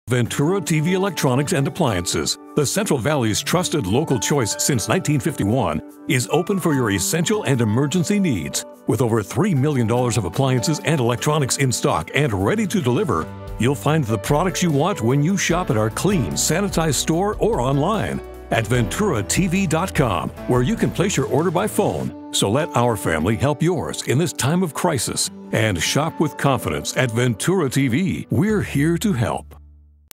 We're Here to Help 30
Client: Ventura TV Video Appliance Center, Inc.
Length: 30s
Description:
Branding TV spot written and produced for Fresno, California based appliance and electronics retailer featuring custom motion graphics, professional voice-overs, composited branded images, stock audio and video.